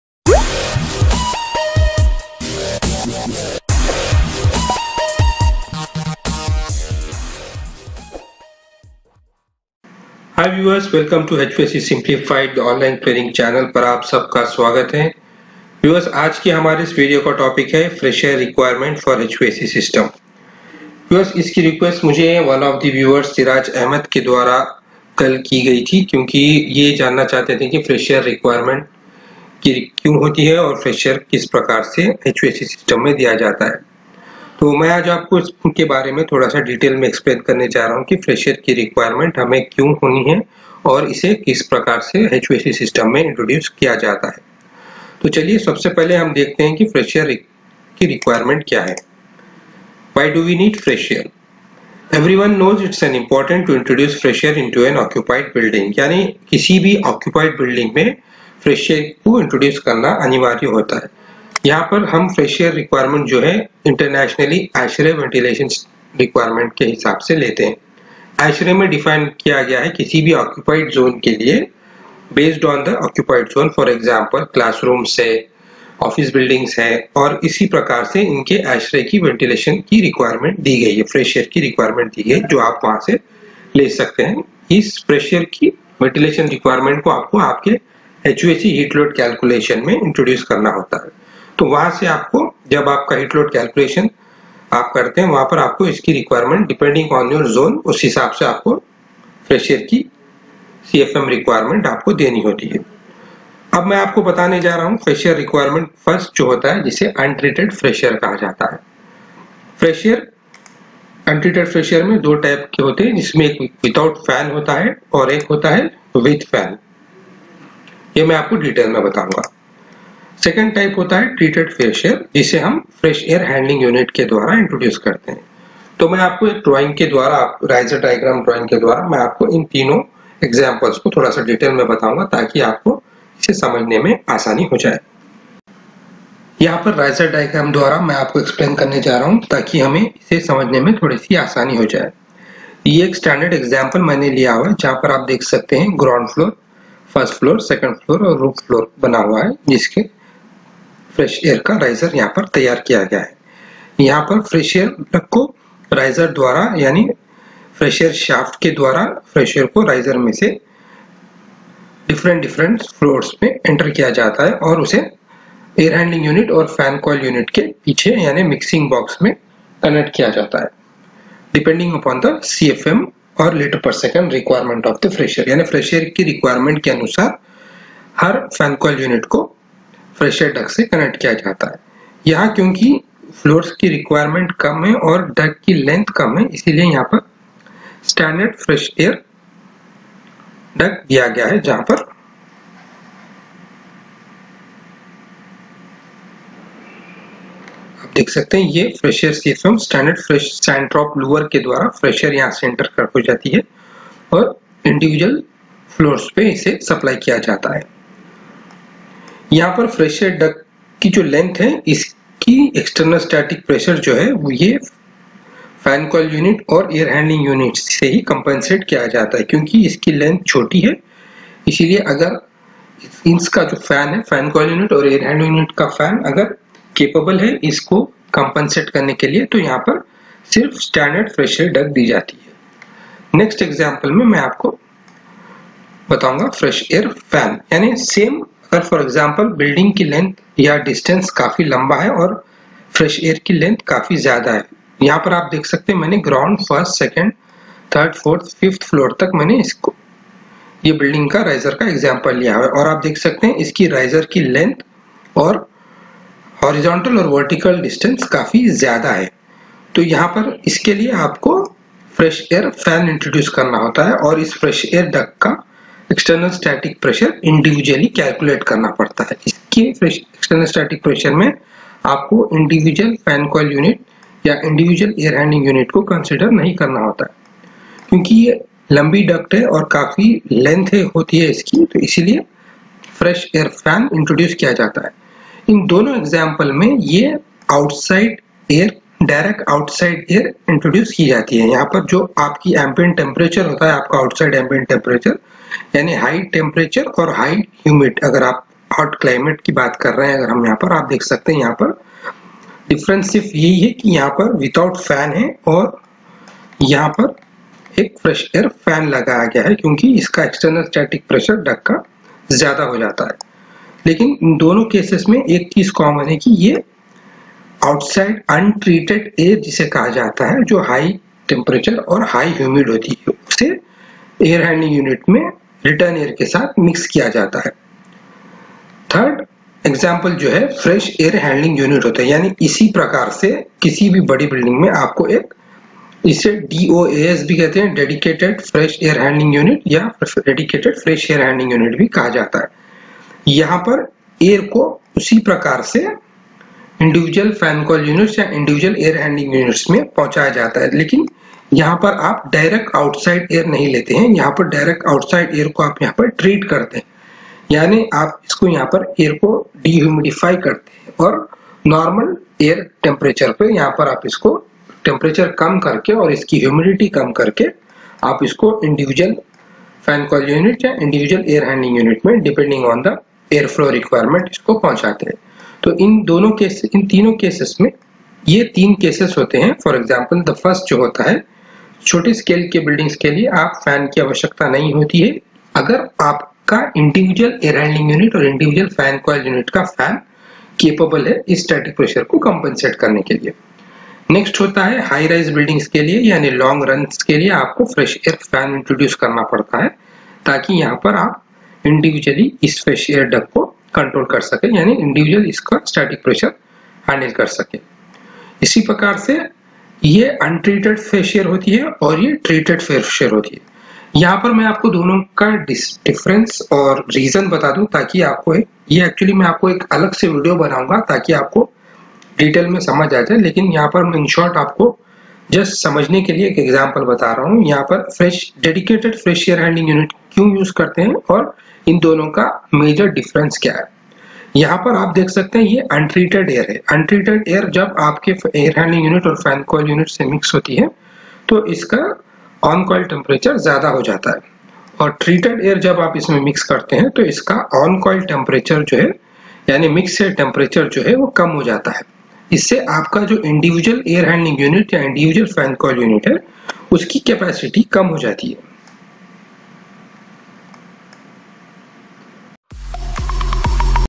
HVAC Training - What is FRESH AIR ..? Explained Untreated Fresh Air VS Treated Fresh Air.
The main difference between TFA and AHU is, TFA conditions 100% outside air and feeds to the building inside whereas an AHU conditions 100% existing air inside a building, or sometimes it takes a few outside airs and mixed with the inside air.